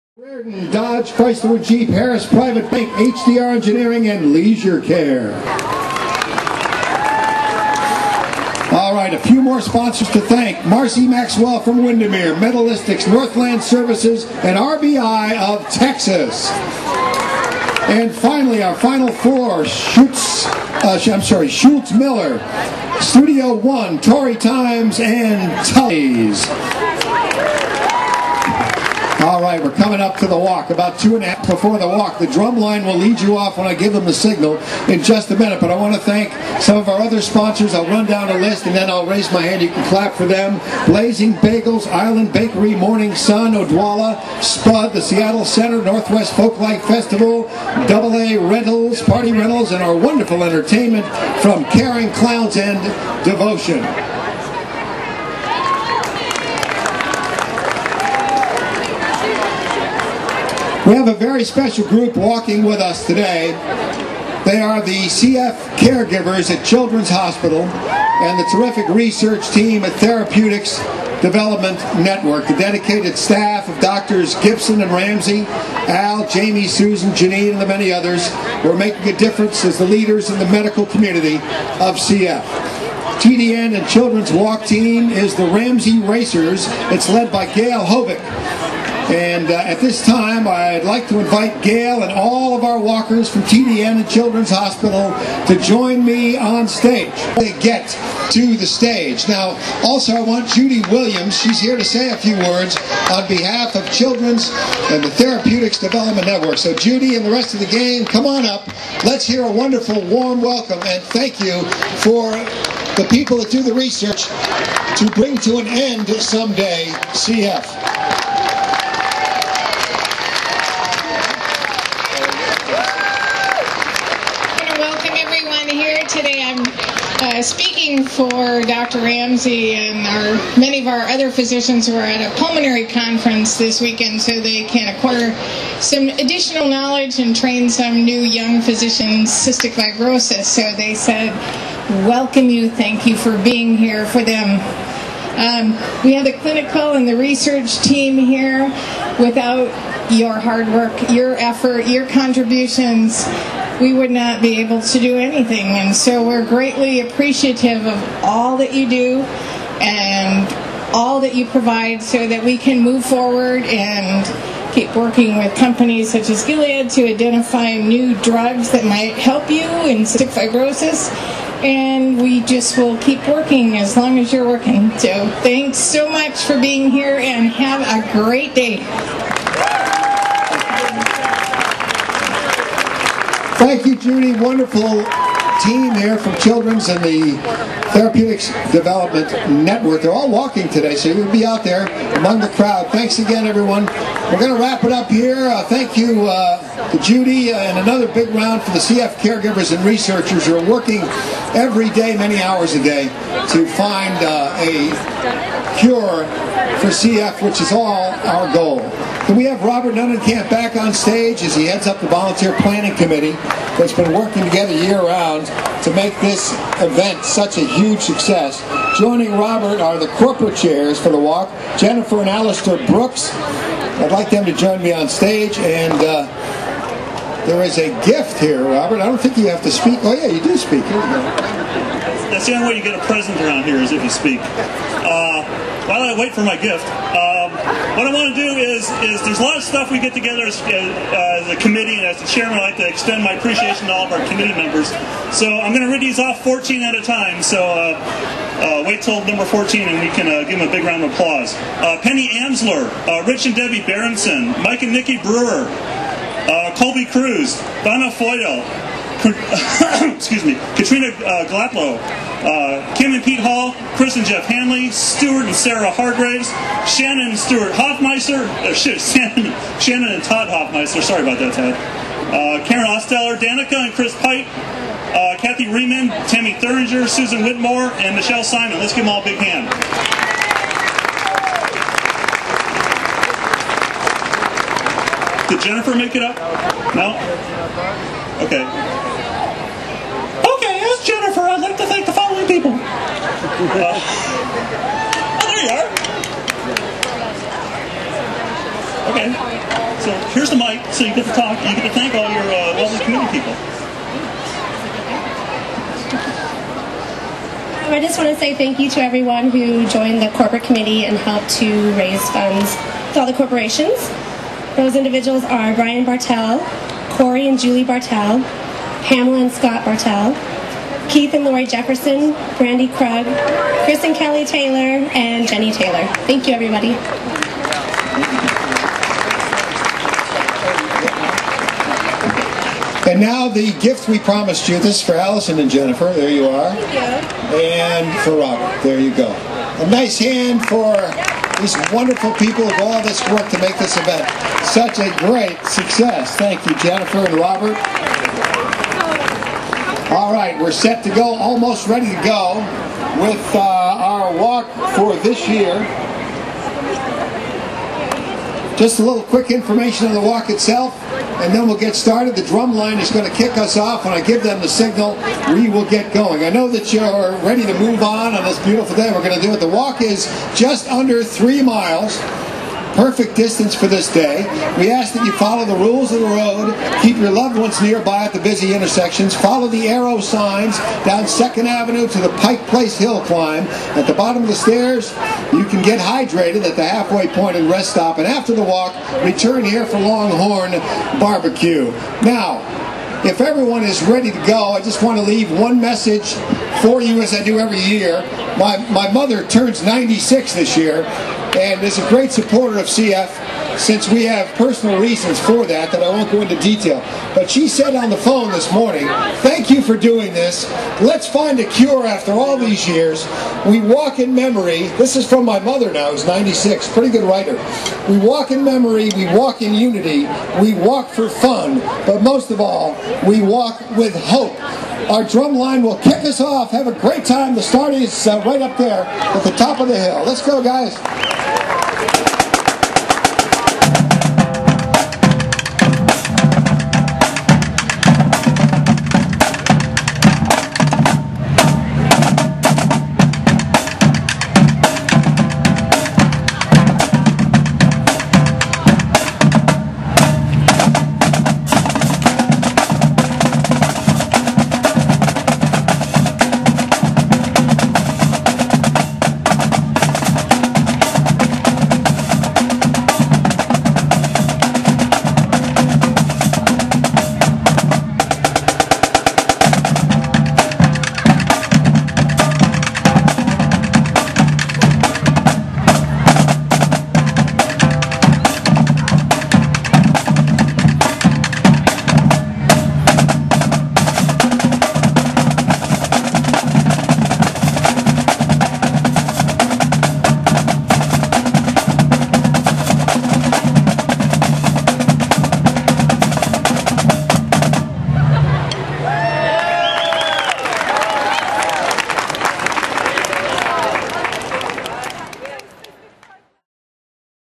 Seattle Great Strides, 5/18/08, Part 2
Part 2 of the pre-walk activities. Over 1,000 walkers raised $413,000 for CF research!